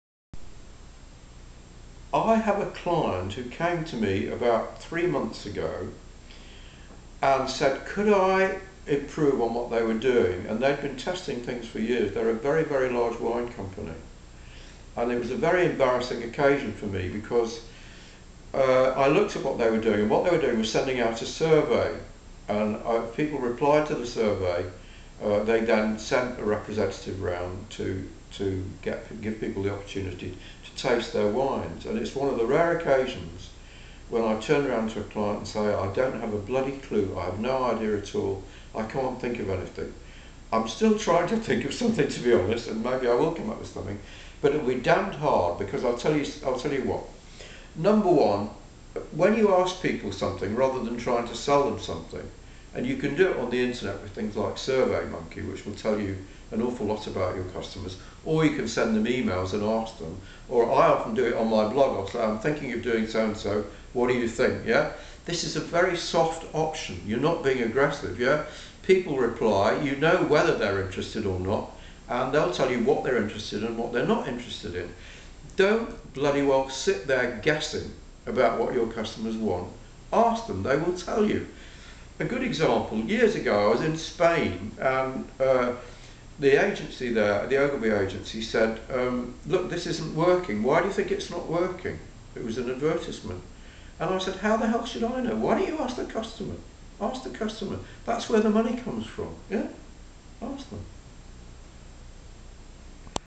Drayton Bird on the power of surveys
Drayton Bird expalins why surveys works so well in direct marketing.